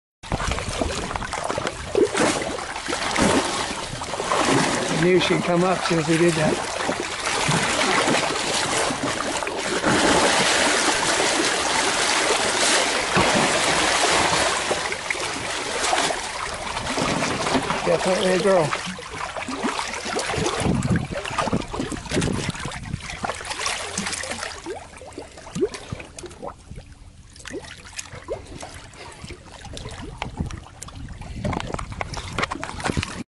Tubarão Branco abocanha carcaça de baleia gigante na Califórnia
Ambientalistas flagraram o momento em que uma grande fêmea de tubarão branco decide fazer um 'lanchinho' e vem abocanhar a carcaça de uma baleia de 16 metros de comprimento que boiava na superfície perto de um pier em Newport Beach, na Califórnia.